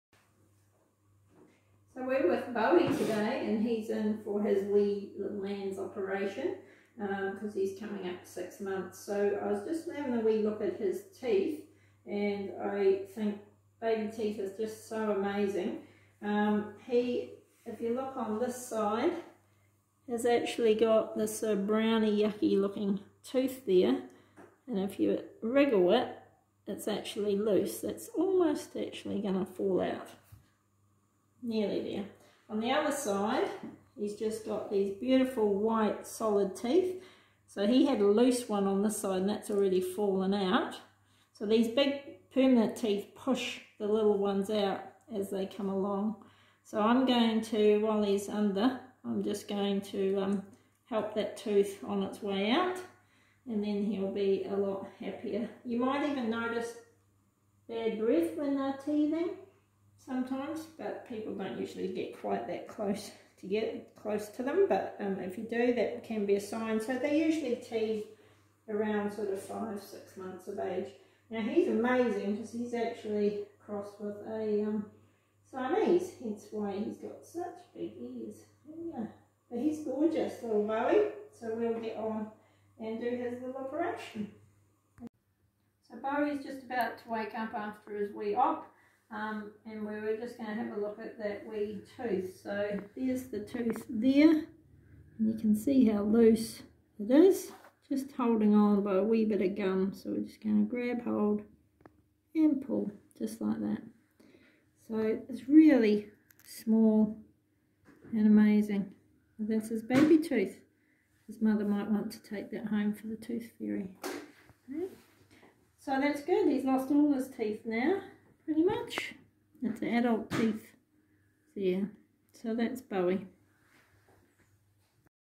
Teething in a cat - Bowie
Picton Vet demonstrates changing of teeth in a young cat around 5-6 months of age. Pet Health Awareness helps pet owners take better care of their pets.